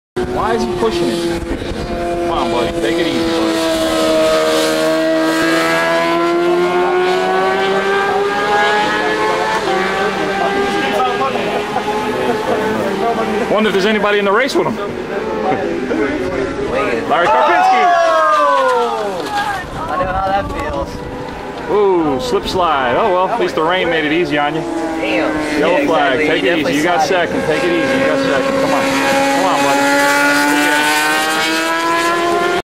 GTU rain race Summit
Turn 10 Summit 1st place goes by and 2nd place slides safe